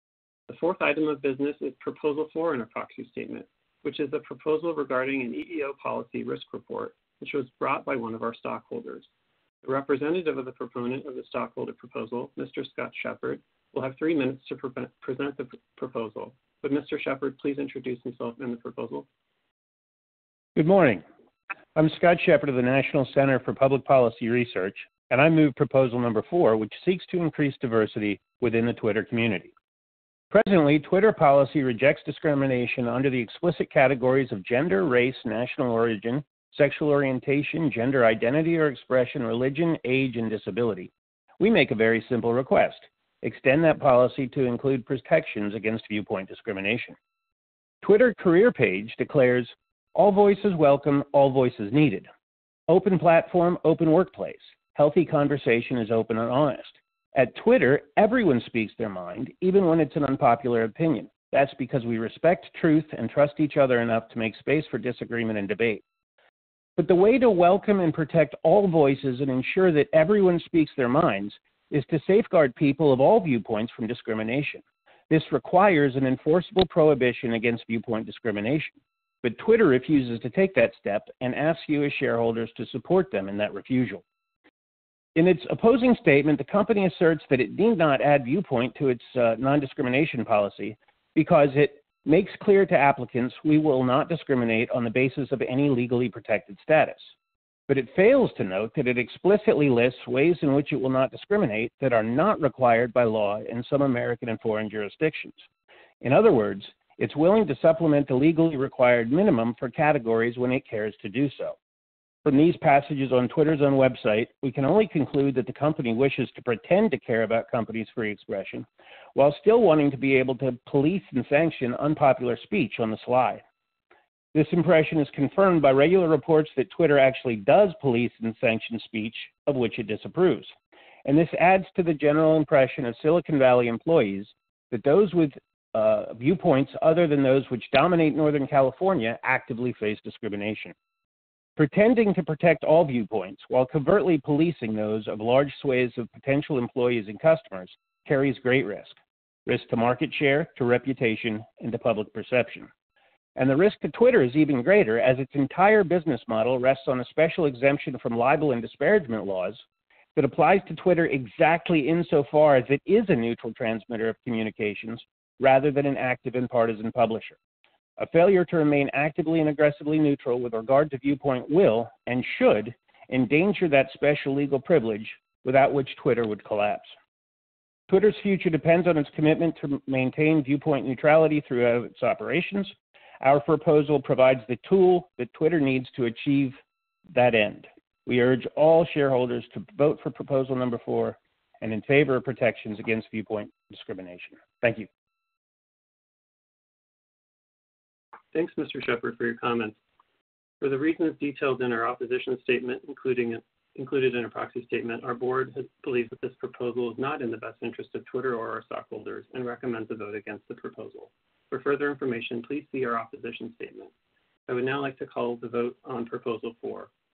Twitter Won't Back up Its Promises of Freedom of Expression
On May 27, 2020, Scott Shepard, Free Enterprise Project Coordinator at the National Center for Public Policy Research, moved Proposal #4 at Twitter's annual shareholder meeting, which sought to increase viewpoint diversity within the Twitter community.

His presentation is here in written and audio format.

Presently, Twitter policy rejects discrimination under the explicit categories of “gender, race, national origin, sexual orientation, gender identity or expression, religion, age [and] disability.” We make a very simple request: extend that policy to include protections against viewpoint discrimination.

Twitter’s Careers page declares: "All voices welcome, all voices needed. Open platform, open workplace. Healthy conversation is open and honest. At Twitter everyone speaks their minds, even when it’s an unpopular opinion. That’s because we respect and trust each other enough to make space for disagreement and debate."

The way to welcome and protect “all voices” and to ensure that “everyone speaks their minds” is to safeguard people of all viewpoints from discrimination. This requires an enforceable prohibition against viewpoint discrimination. But Twitter refuses to take that step, and asks you as shareholders to support them in that refusal.

In its opposing statement, the company asserts that it need not add viewpoint to its non- discrimination policy because it “make[s] clear to applicants that we will not discriminate on the basis of any legally protected status.” But it fails to note that it explicitly lists ways in which it will not discriminate that are not required by law in some American and foreign jurisdictions. In other words, it is willing to supplement the legally required minimum for categories when it wants to do so.

From these passages on Twitter’s own website, we can only conclude that the company wishes to pretend to care about employees’ free expression, while still wanting to be able to police and sanction unpopular speech on the sly.

This impression is confirmed by regular reports that Twitter actually does police and sanction speech of which it disapproves. And this adds to the general impression of Silicon Valley employees that those with viewpoints other than those which dominate Northern California actively face discrimination.

Pretending to protect all viewpoints, while covertly policing those of large swathes of potential employees and customers, carries great risks: risks to market share, to reputation and to public perception. And the risk to Twitter is even greater, as its entire business model rests on a special exemption from libel and disparagement laws that apply to Twitter exactly insofar as it is a neutral transmitter of communications rather than an active and partisan publisher. A failure to remain actively and aggressively neutral with regard to viewpoint will – and should – endanger that special legal privilege without which Twitter would collapse.

Twitter’s future depends on its commitment to maintain viewpoint neutrality throughout its operations. Our Proposal provides the tool that Twitter needs in order to achieve that. We urge all shareholders to vote for Proposal #4, and in favor of protections against viewpoint discrimination.